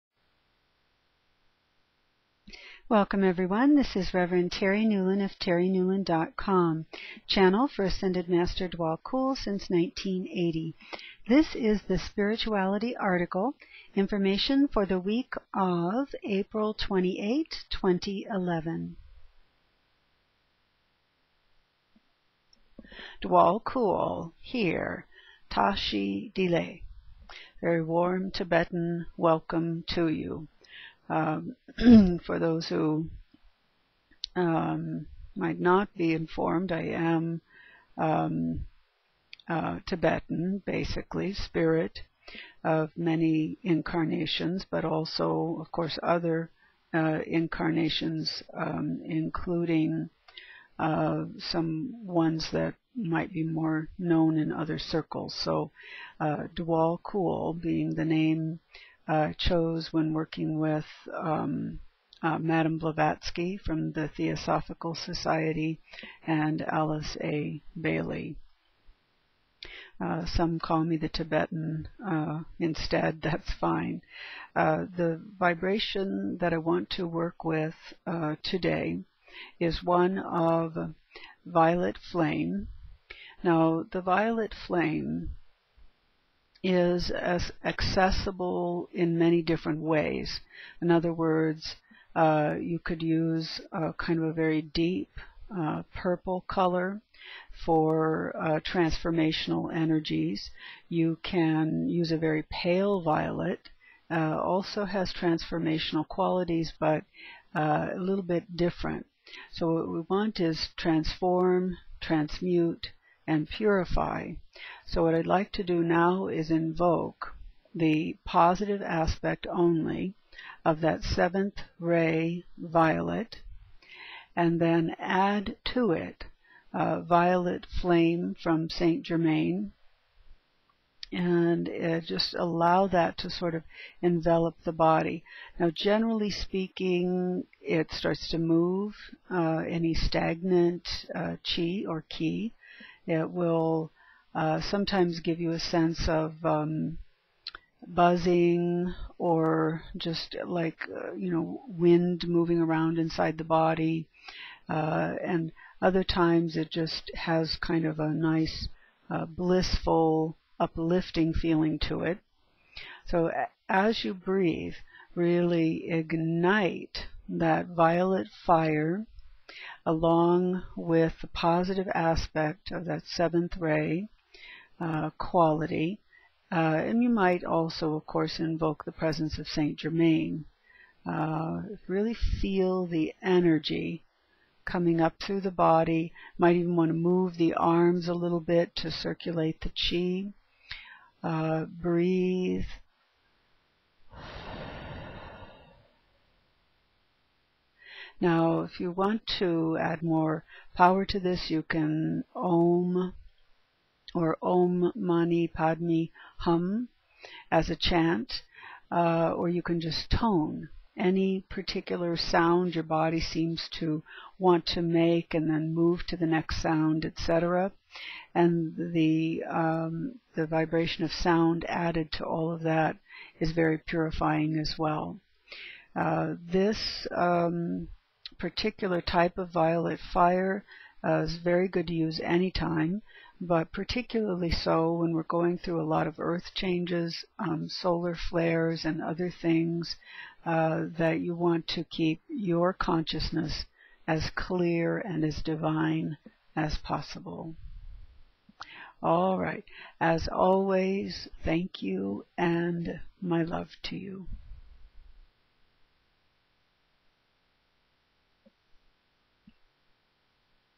VioletFirePositiveAspect20110428DKtnWSAV.wmv
Use of positive aspect of 7th Ray with St. Germaine violet fire; Channeled Teachings by Ascended Master Djwhal Khul through Rev. Terri Newlon since 1980